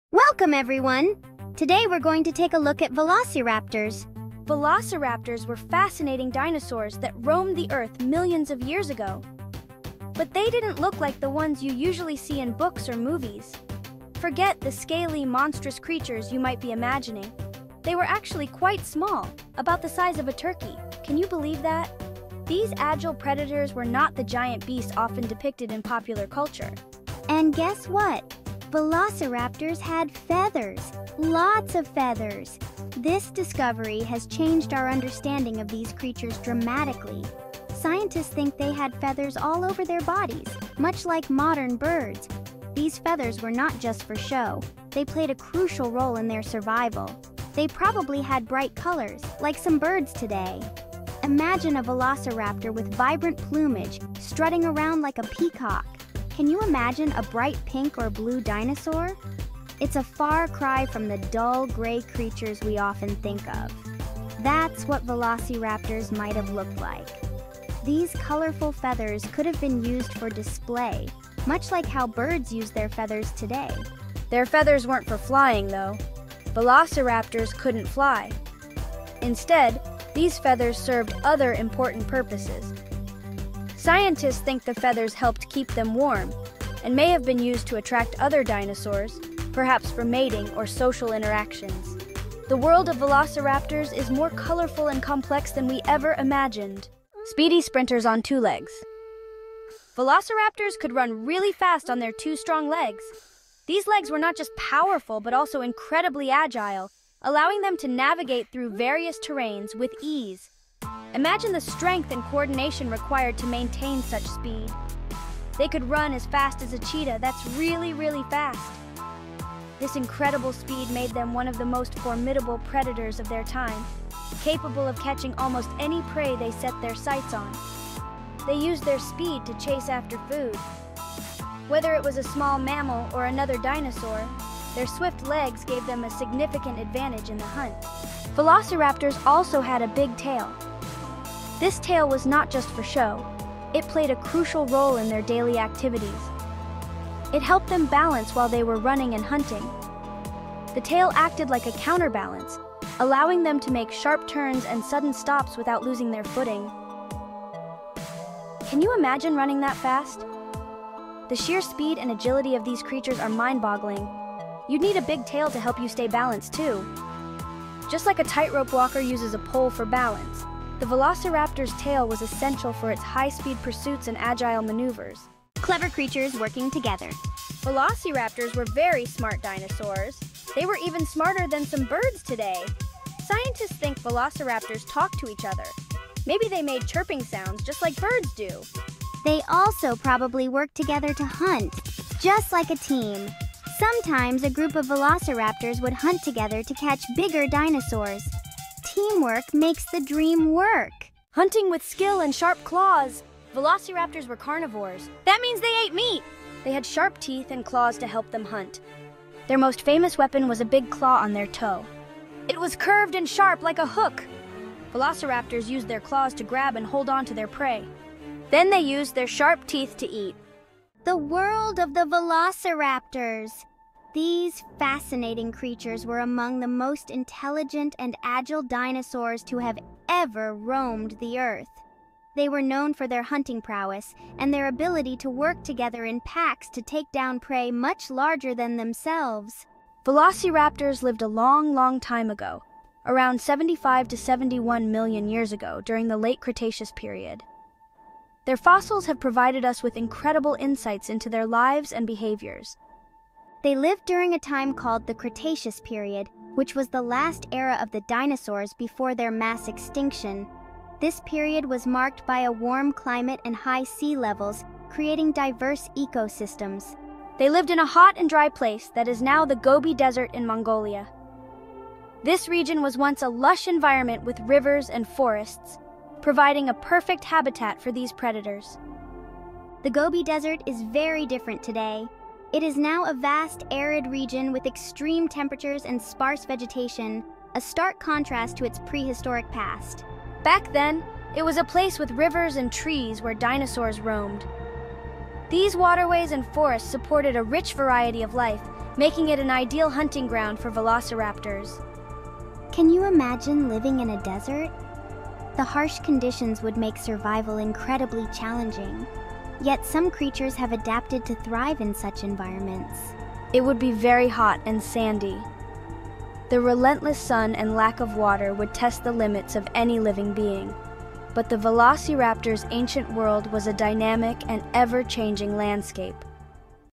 "Raptors" Velociraptors Dinosaurs: The Real Dino Story!
Discover the Fascinating World of Velociraptors! 🦖🌟 In this thrilling video, we debunk common myths and uncover astonishing facts about these incredible creatures. Learn how their feathers weren't for flying, but for warmth and attraction, and how they were more colorful than Hollywood suggests. Despite their small size – similar to a turkey – Velociraptors boasted remarkable speed, akin to a cheetah, which they used to hunt in packs with impressive intelligence and communication skills. Delve into their carnivorous diet, sharp teeth, and powerful claws, and envision their life in the lush, ancient Gobi Desert during the Cretaceous period.

OUTLINE:
00:00:00
A Look at Velociraptors

00:01:37
Speedy Sprinters on Two Legs

00:02:55
Clever Creatures Working Together

00:03:21
Hunting with Skill and Sharp Claws

00:03:40
The World of the Velociraptors